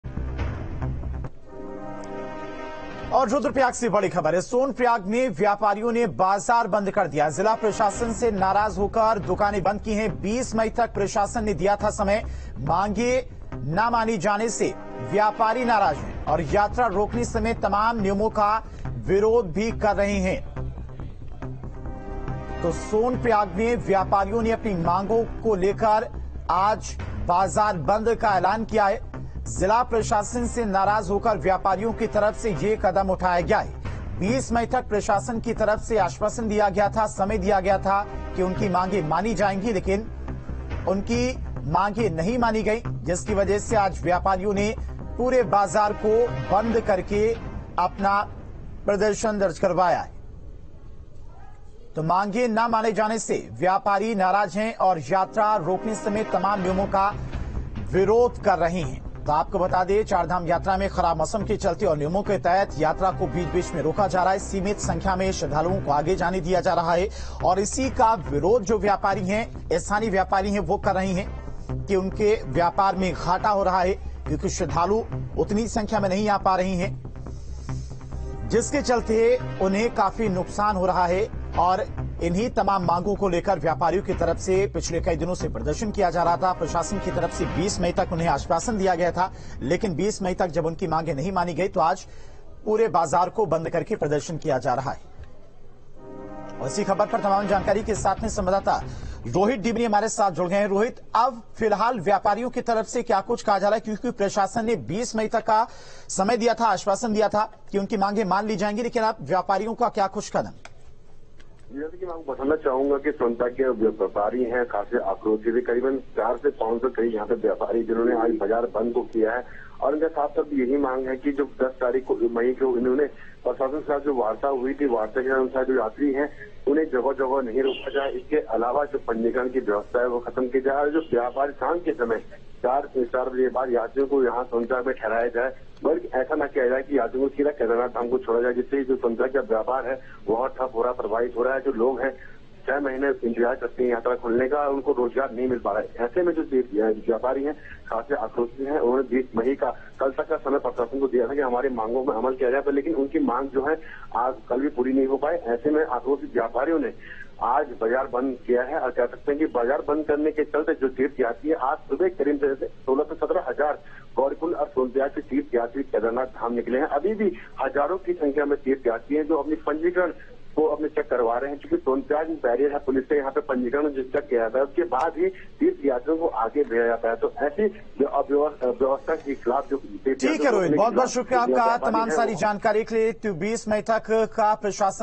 Kedarnath Dham: सोनप्रयाग में व्यापारियों ने किया बाजार बंद, मांगे न पूरी होने से व्यापारी नाराज ।
News State Apps डाउनलोड करें:
Download Android and iOS Apps for more updates: